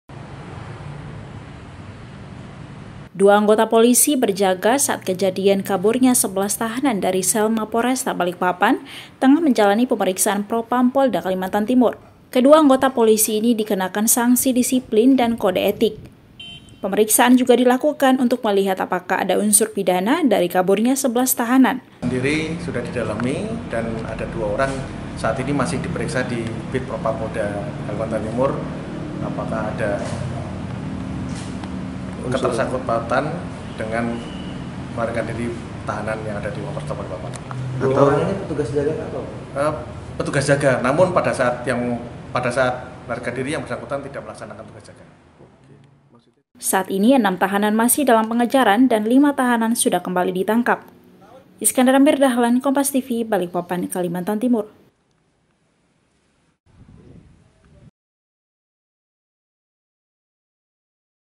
2 Anggota Polisi Diperiksa Propam Polda Kaltim Pasca 11 Tahanan Kabur dari Sel
BALIKPAPAN, KOMPAS.TV  Dua anggota polisi yang berjaga saat kejadian kaburnya 11 tahanan dari sel Mapolresta Balikpapan, tengah menjalani pemeriksaan Propam Polda Kalimantan Timur.

Kedua anggota polisi ini dikenakan sanksi disiplin dan kode etik. Pemeriksaan juga dilakukan untuk melihat apakah ada unsur pidana dari kaburnya 11 tahanan.

Saat ini 6 tahanan masih dalam pengejaran dan 5 tahanan sudah kembali ditangkap.(*)